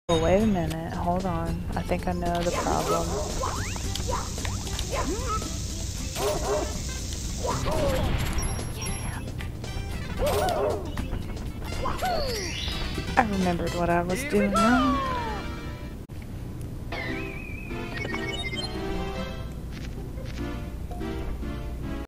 Highlight: Clippy!
Just a little highlight of the Bomb Clip speedruning strategy that I've recently learned.